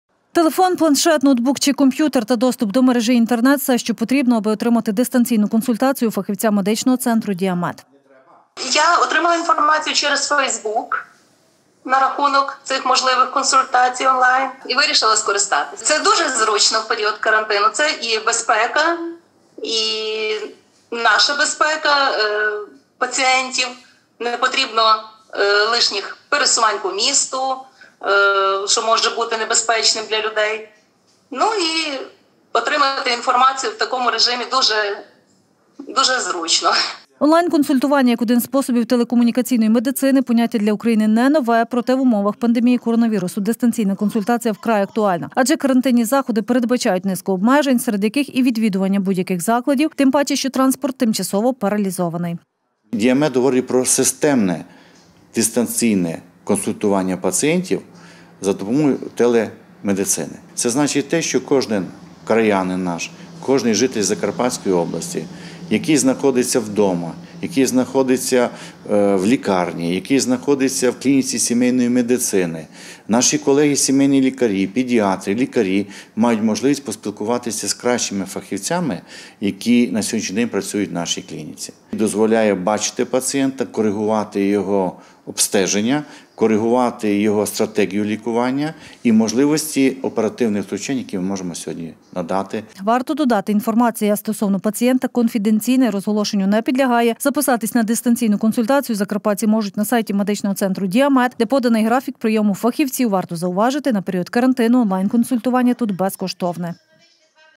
Онлайн консультування
Телекомунікаційна медицина успішно використовується на теренах України, проте особливої актуальності набула під час пандемії коронавірусу, коли громадський транспорт та міжміське сполучення призупинене, відвідування будь - яких закладів обмежене та діють карантинні норми, порушувати які – зась! Системне безкоштовне консультування онлайн  під час карантину запровадив і один з провідних медичних центрів Ужгорода  -«Діамед».
Телефон, планшет, ноутбук чи комп’ютер та  доступ до мережі Інтернет – все, що потрібно, аби отримати дистанційну консультацію фахівця  медичного центру «Діамед». Онлайнконсультування - як один способів телекомунікаційної медицини – поняття для України не нове! Проте в умовах пандемії коронавірусу, дистанційна консультація вкрай актуальна! Адже карантинні заходи передбачають низку обмежень, серед яких і відвідування будь яких закладів, тим паче, що транспорт тимчасово паралізований. Варто додати, інформація стосовно пацієнта –конфіденційна і розголошенню не підлягає! Записатися на дистанційну консультацію закарпатці можуть на сайті медичного центру «Діамед», де поданий графік прийому фахівців. Варто зауважити, на період карантину онлайн консультування тут -БЕЗКОШТОВНЕ!